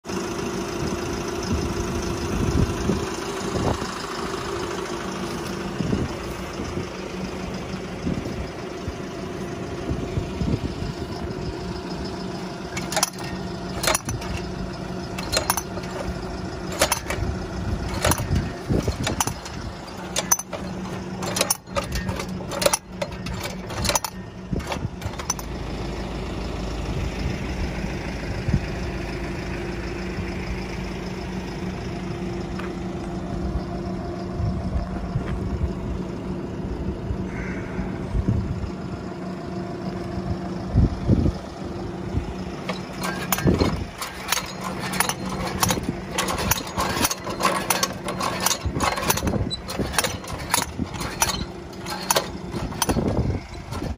Mechanical 4000 transplanter
Quick look on Mechanical 4000 transplanter for cell type plants.